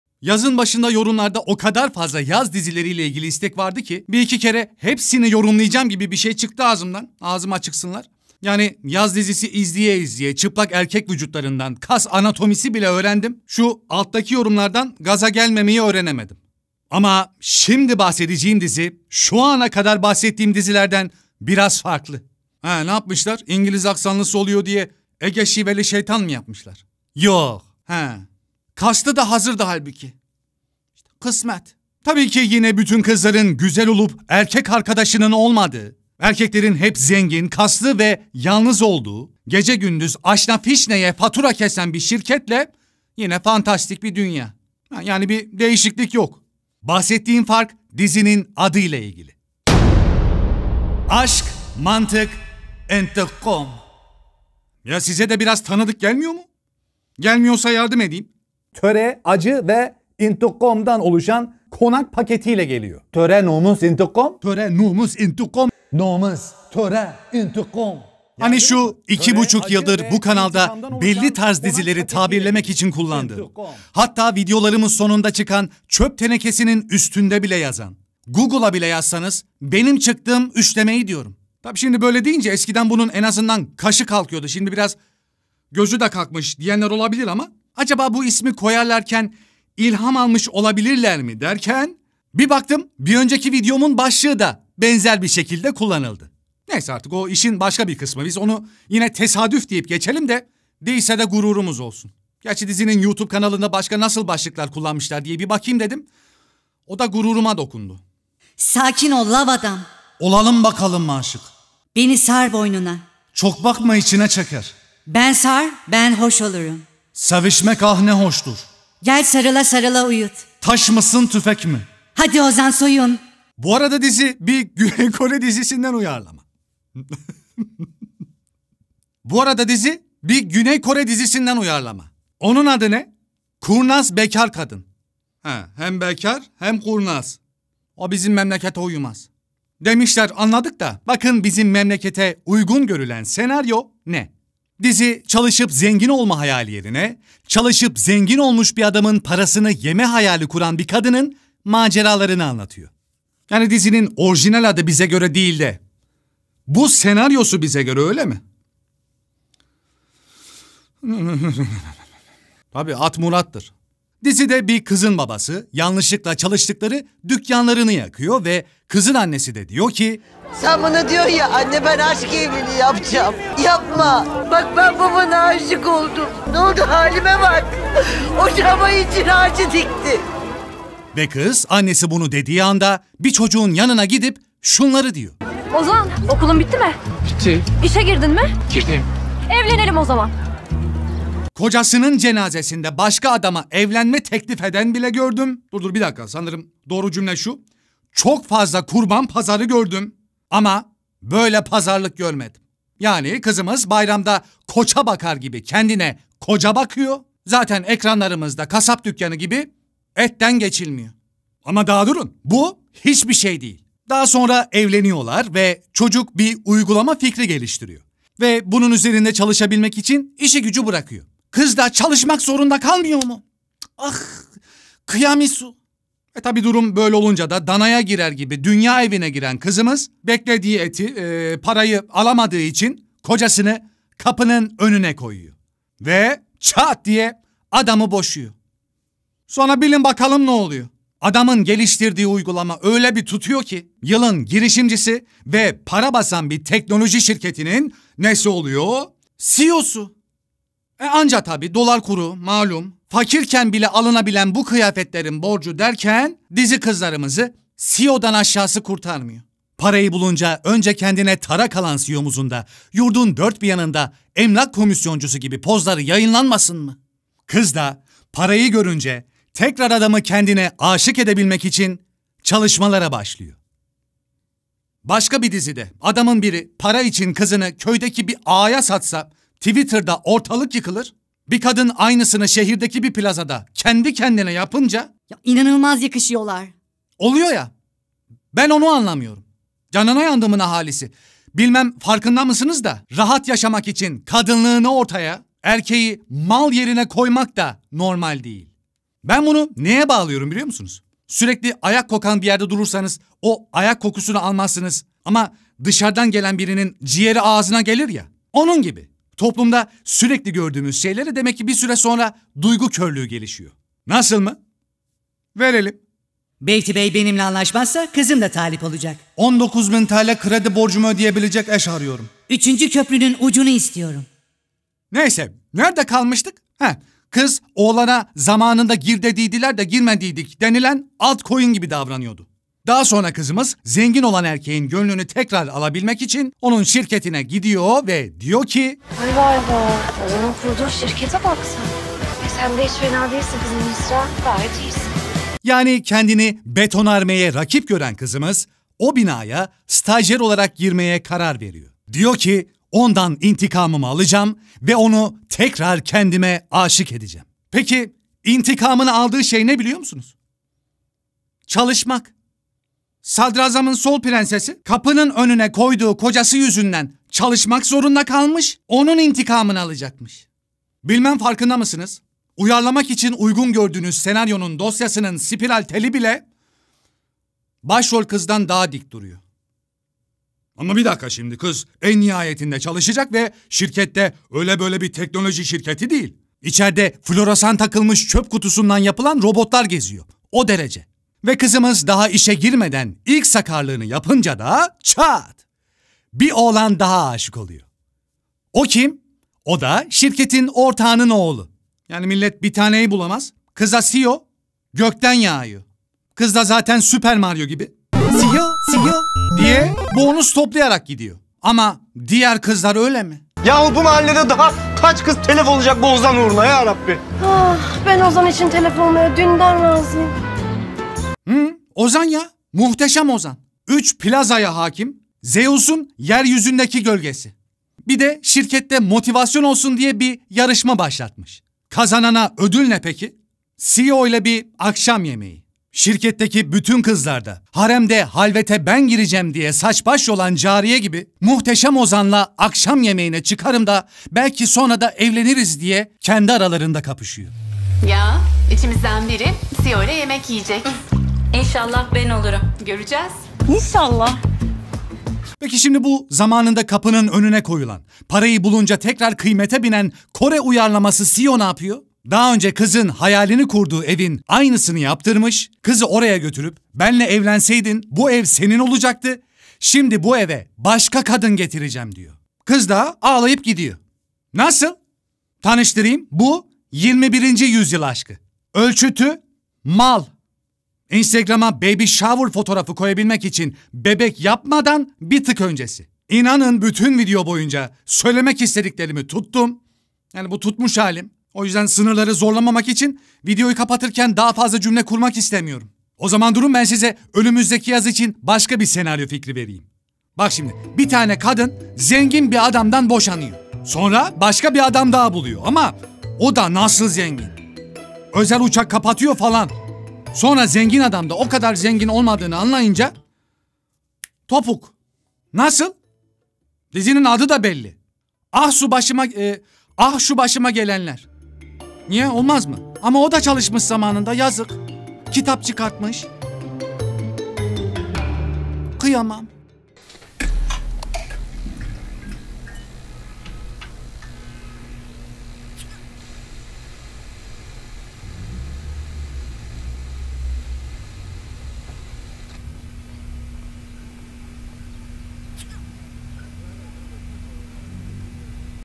AŞK MANTIK İNTİKAM Dizi İncelemesi - Mal Mülk İntikam
Aşk Mantık İntikam, 2.5 yıldır kanalda belli türkdeki dizileri tabir ederken kullandığın "töre, nomıs, intikom" üçlemesini andırdığı için üzerimde beklentisi oluşan bir videoydu. O yüzden daha fazla bekletemedim. Ama bir şey demek istiyorum, ben yaz dizisi izlemekten çok sıkıldım.

Daha önce bahsettiğim 2021 yaz dizilerinin incelemelerine de şöyle gidiliyor;